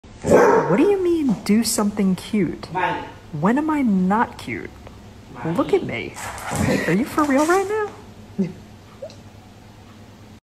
I’m always cute!
I'm always cute! Pumpkin hates sharing his toys.